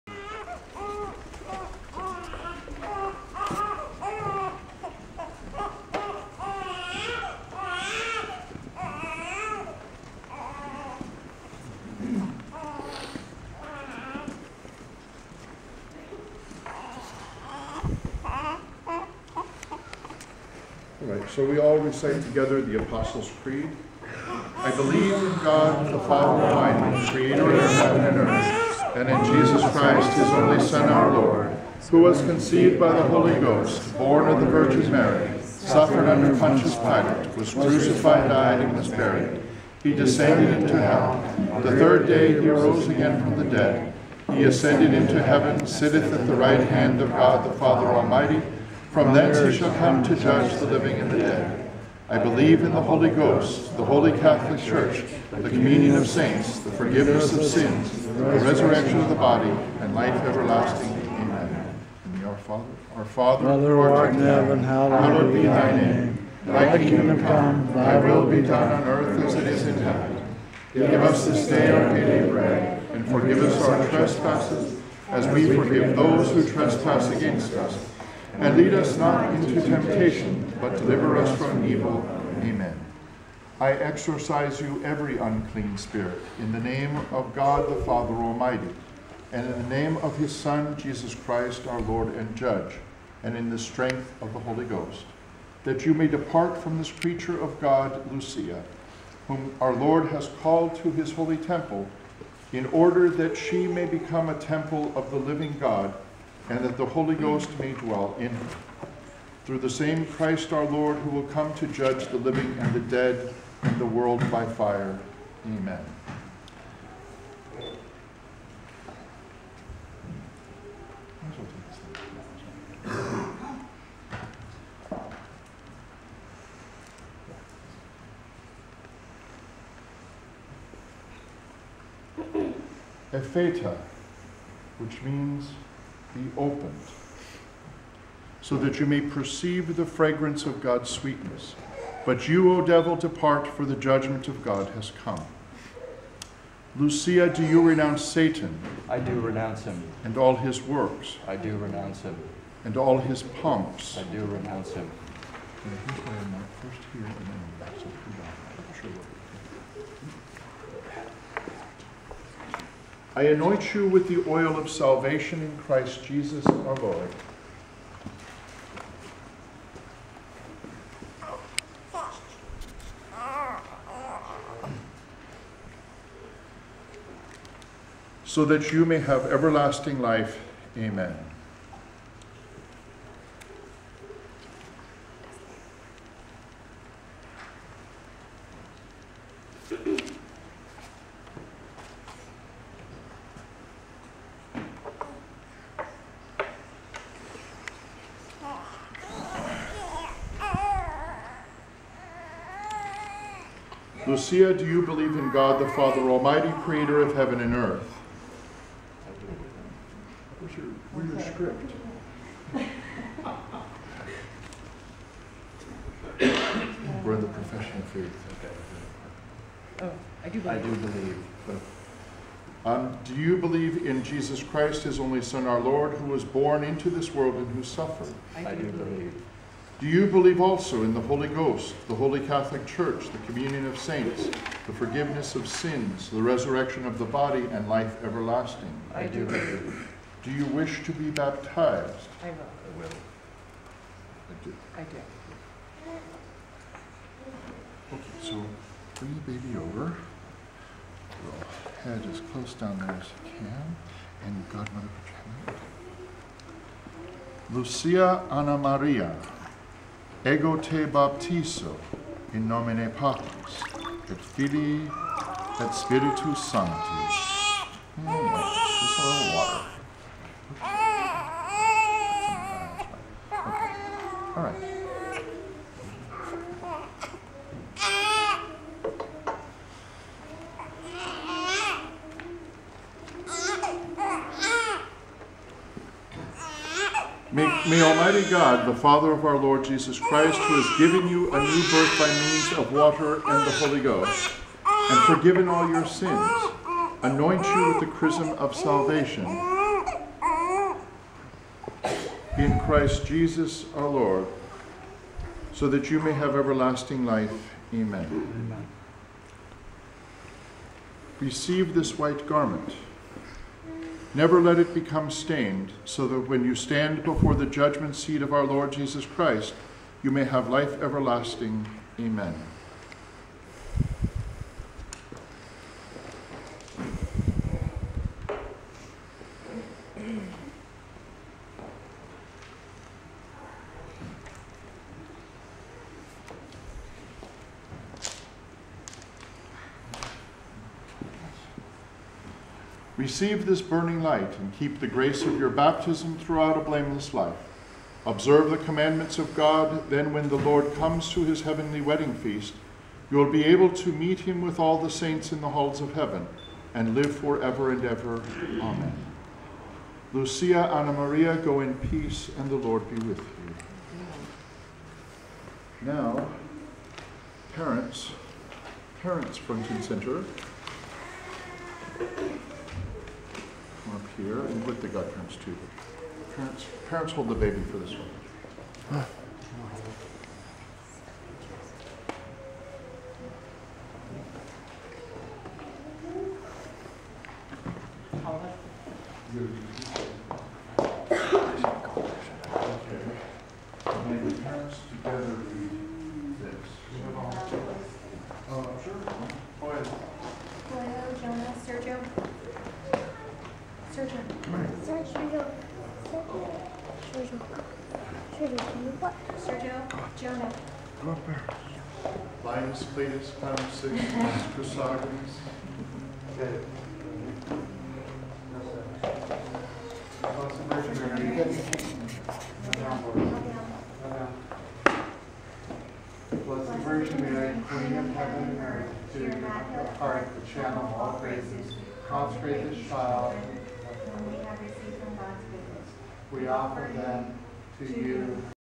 Traditional Catholic Baptism - Lucia Anna Maria (Part II)
MVI 1549.  The rubrics of these baptismal rites date back to the 4th century.  For more information on these rites, you can go to fisheaters.com.